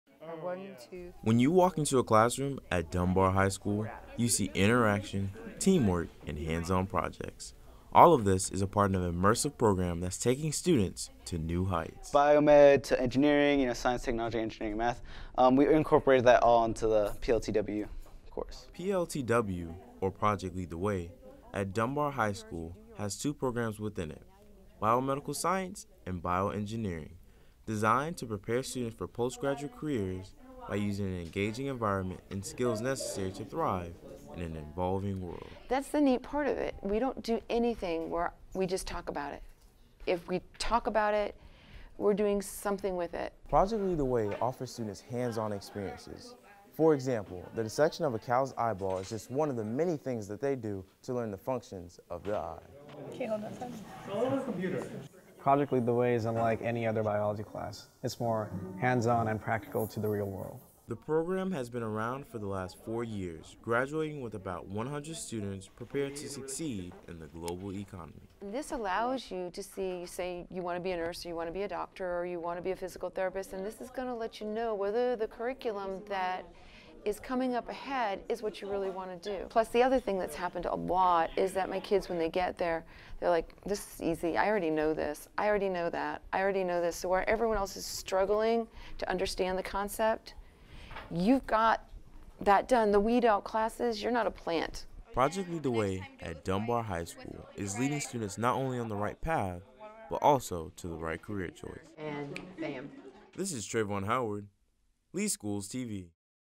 Leading The Way
Project Lead The Way provides a comprehensive approach to STEM Education.  Students at Dunbar High School are given the chance to apply what they know, identify problems, find unique solutions, and lead their own learning. Produced by LeeSchools TV Student Intern Tre'Vaughn Howard.